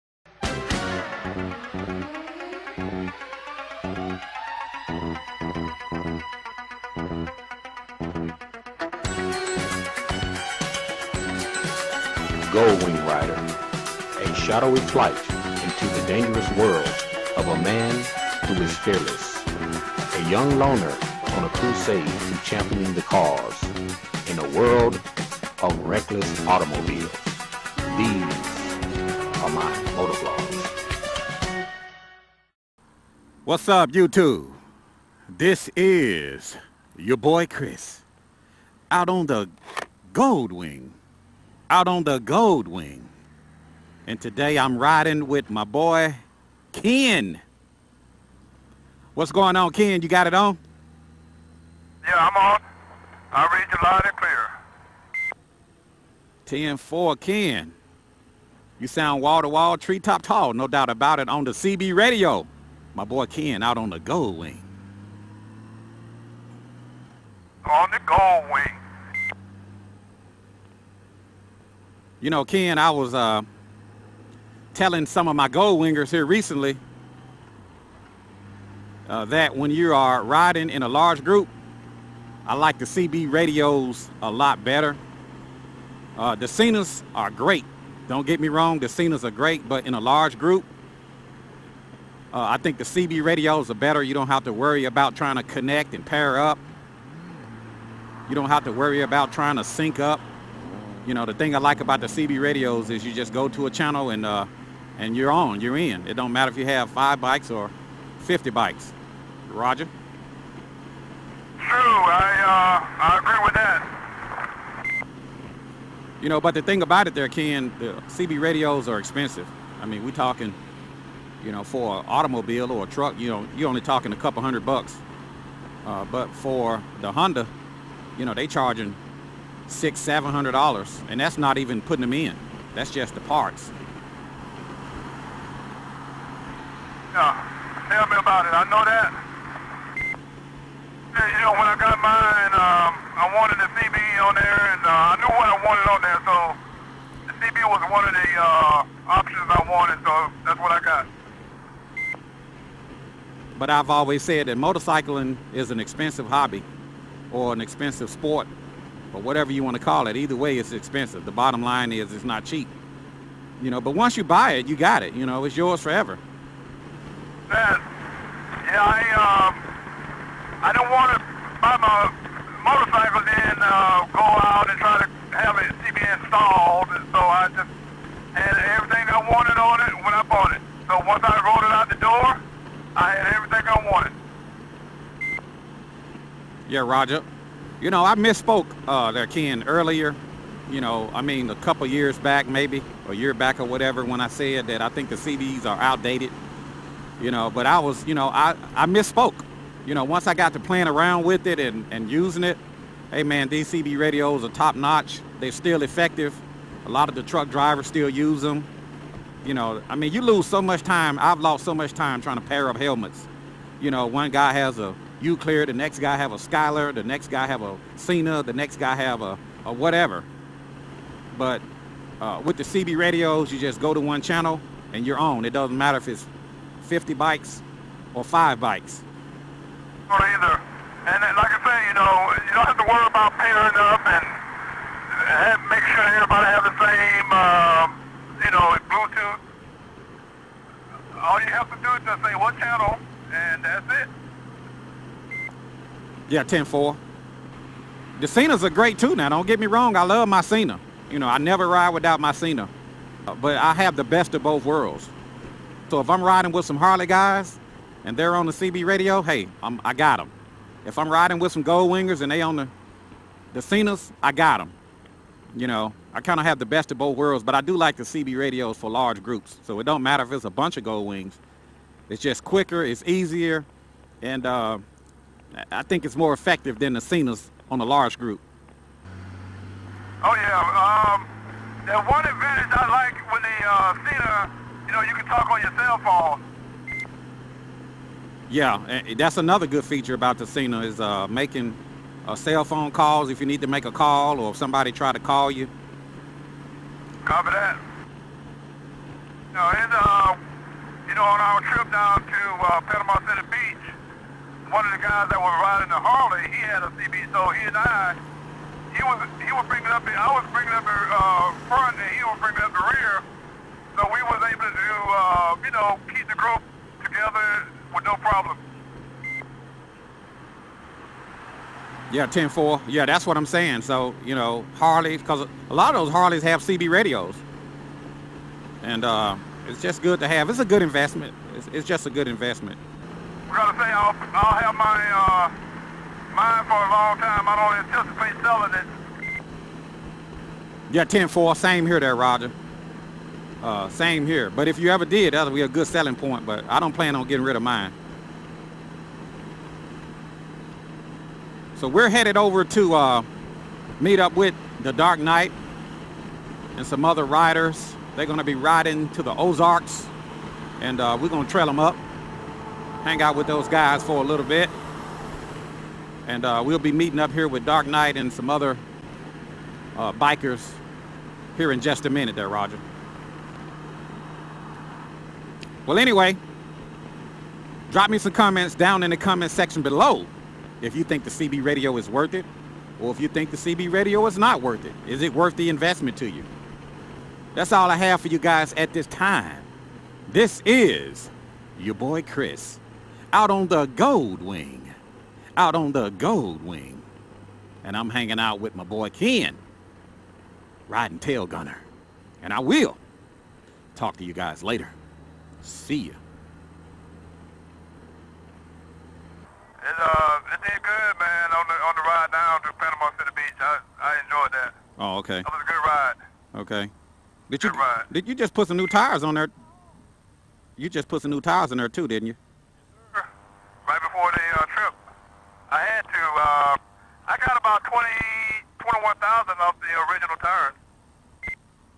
Goldwing ride with Ken while talking CB radios
Goldwing ride with Ken to meet up with The Dark Knight.